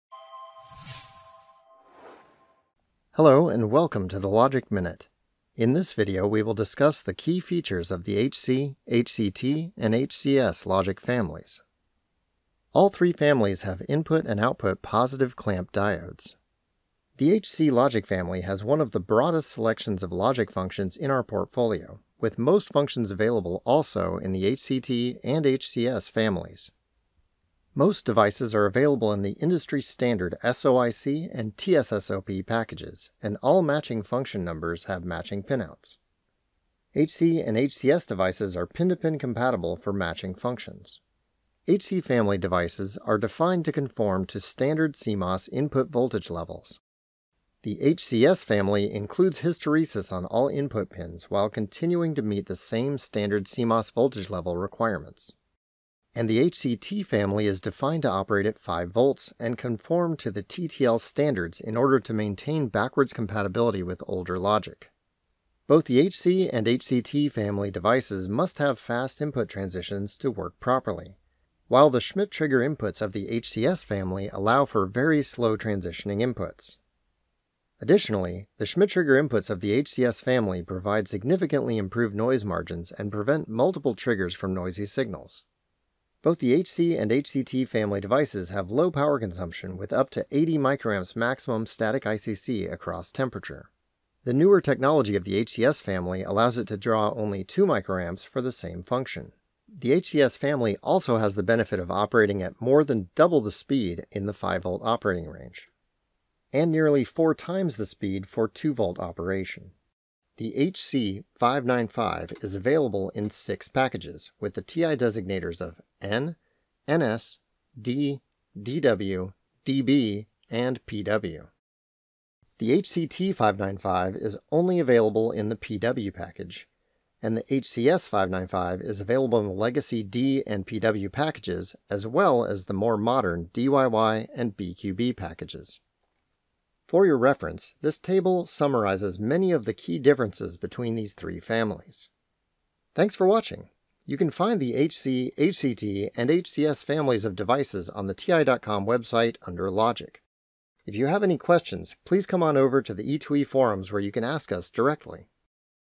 HC, HCT, and HCS Logic family overview
This video goes over the similarities and differences for the HC, HCT, and HCS families of logic.
View our logic portfolio and technical resources.